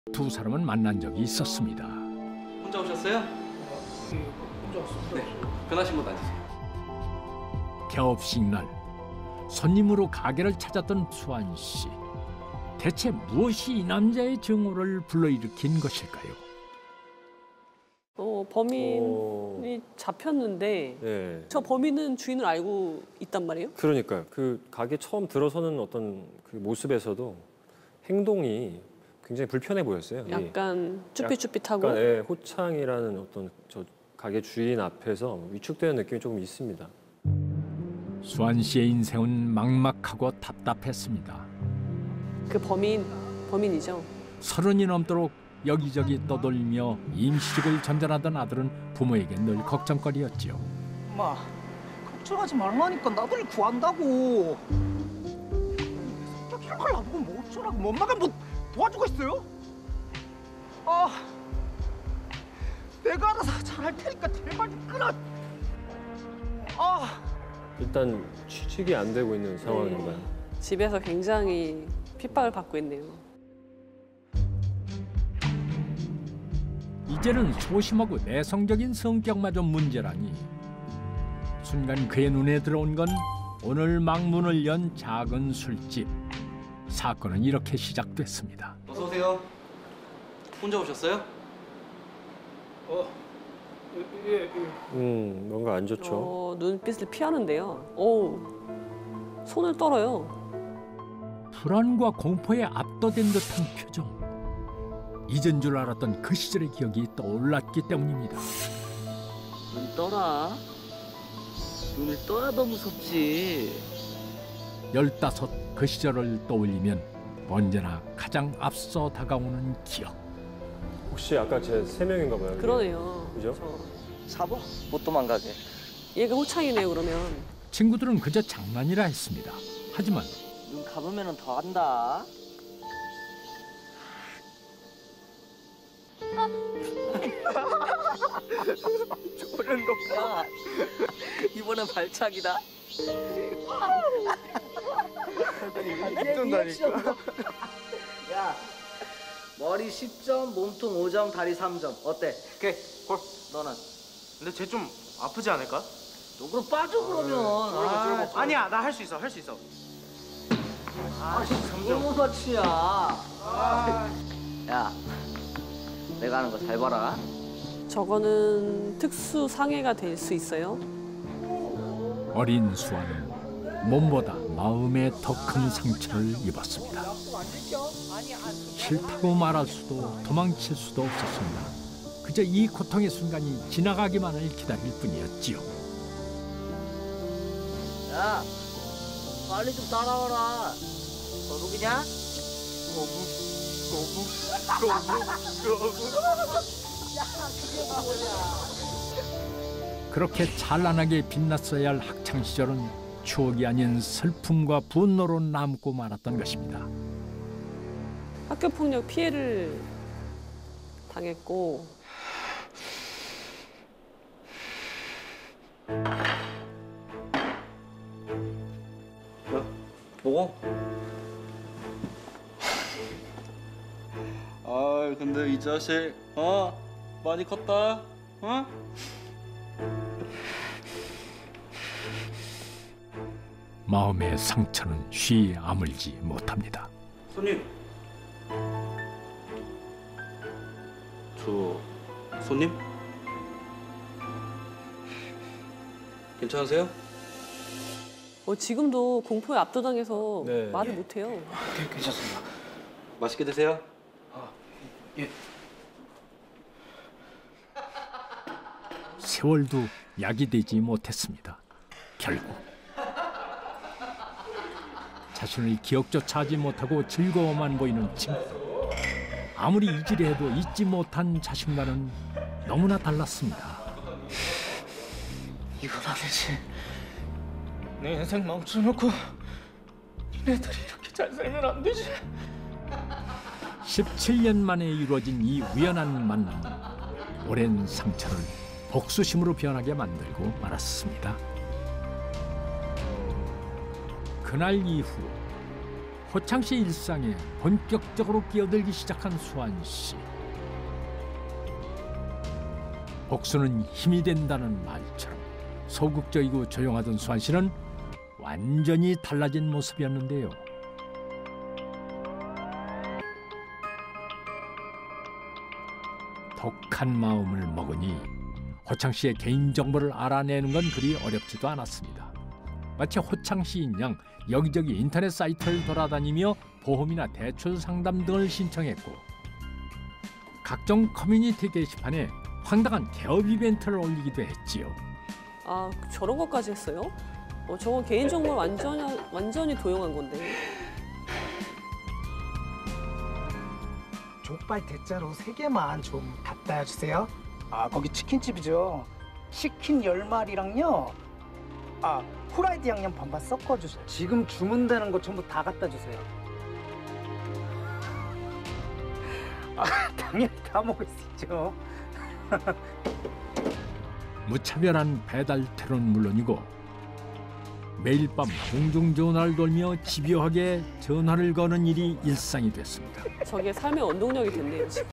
[※사건기록※]드디어 잡힌 범인, 그의 정체는? MBN 210824 방송
6개월 전 개업식 날 가게를 방문했던 범인. 그런데 그의 행동이 부자연스러운 이유는?
사장과 그 동창들에게 지속적으로 학교폭력을 당했던 범인은
행복해보이는 그들의 모습에 주체할 수 없는 증오를 느꼈던 것.